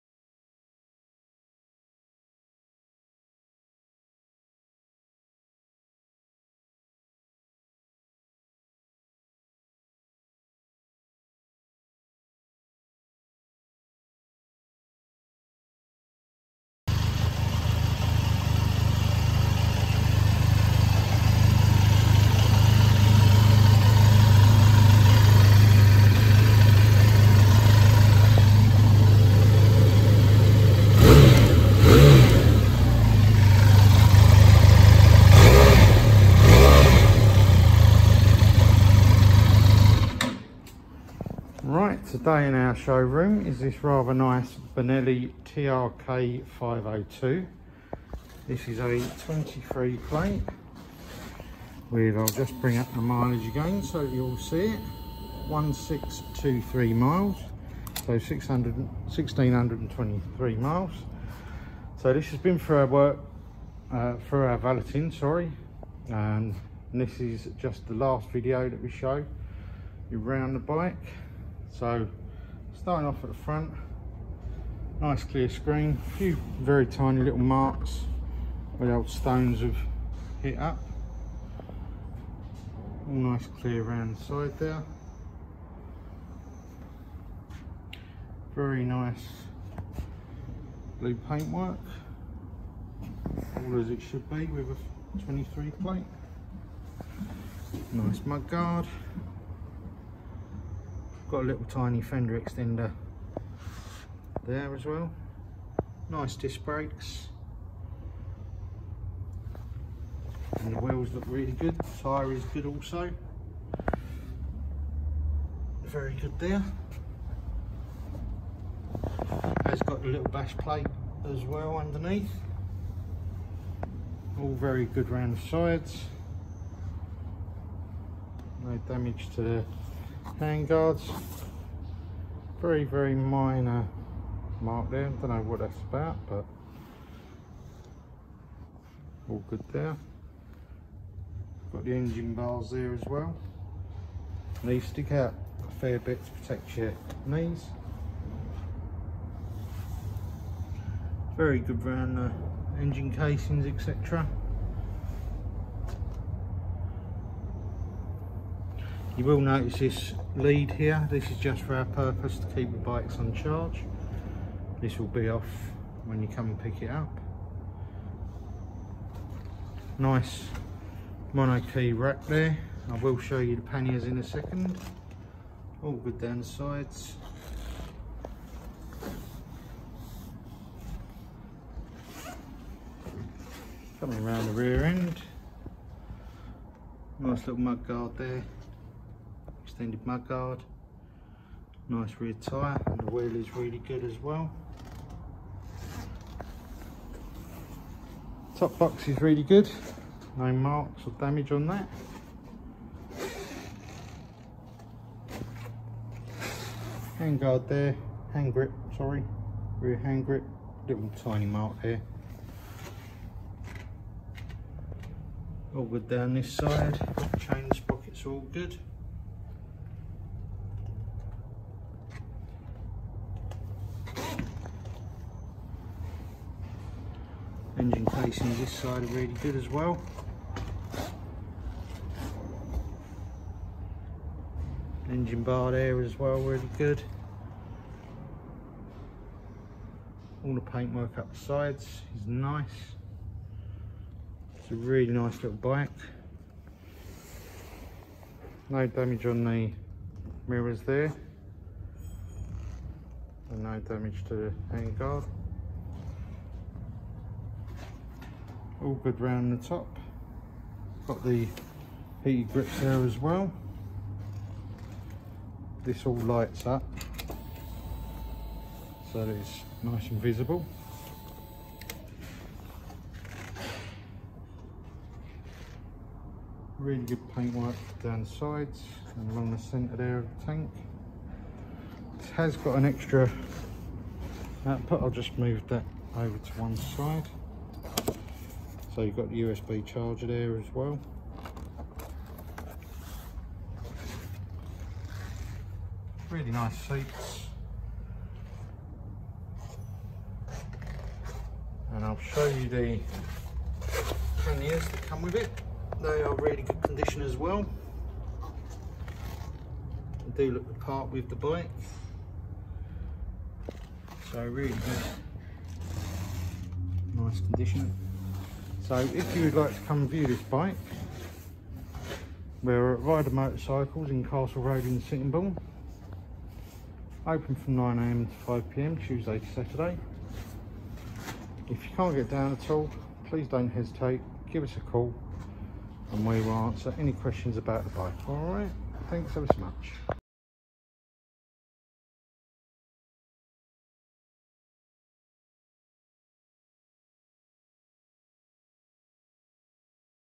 2023 Benelli TRK 502 - Used Bike Video Tour
We have this 2023 Benelli TRK502 in stock now. Come and view it for yourself in store or buy